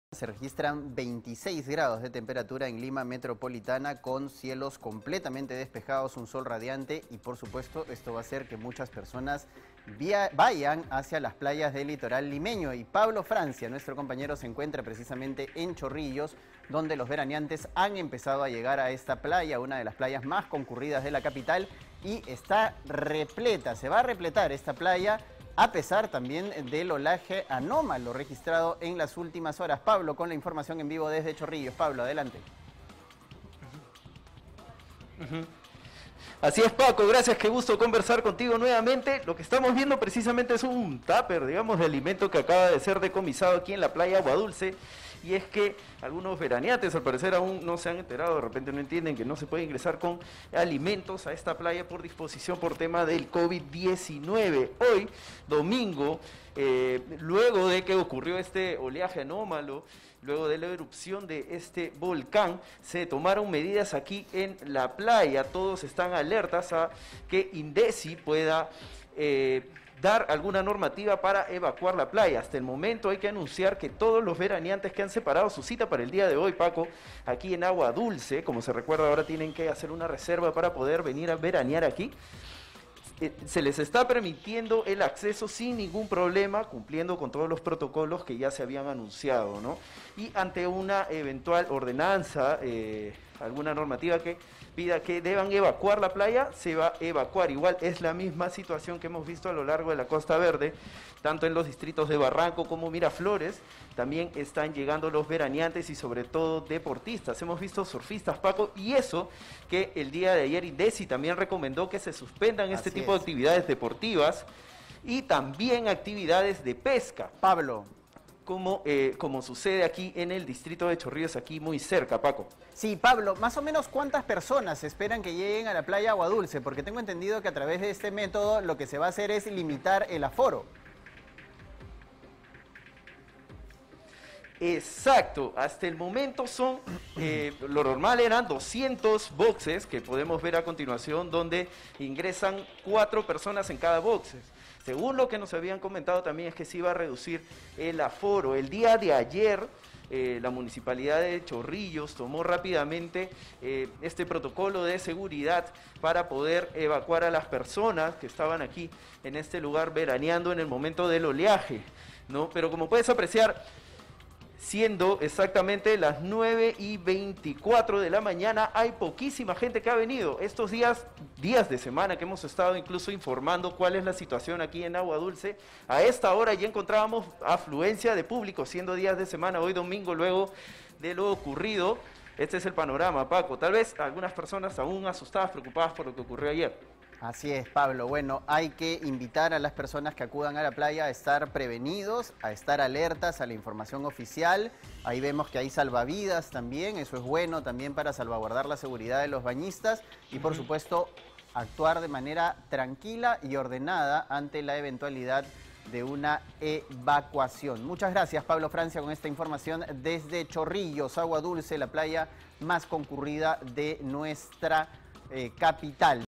Costa Verde: bañistas acuden a las playas pese a oleajes anómalos registrados en la víspera
Esta mañana, decenas de bañistas llegaron a las distintas playas de la Costa Verde pese a los fuertes oleajes anómalos registrados en la víspera, que incluso dejaron dos mujeres muertas en Lambayeque. Ese fue el caso de la playa Agua Dulce en Chorrillos.

Hombres, mujeres y niños pasaban por los controles de seguridad, muy entusiasmados por disfrutar del mar. Personal de la comuna revisaba mochilas, maletines y bolsas para evitar el ingreso de alimentos y bebidas alcohólicas.

Las autoridades señalan que con el paso de las horas llegará más público al balneario chorrillano, pero solo podrán ingresar las personas que separaron sus cupos con anterioridad a través de la página web del municipio.

Erupción volcánica en Tonga

Una erupción volcánica submarina en el Pacífico desató el pánico en las islas Tonga, con un tsunami que provocó olas de 1,20 metros en Japón y generó alertas en la costa oeste de Estados Unidos, Ecuador y Chile.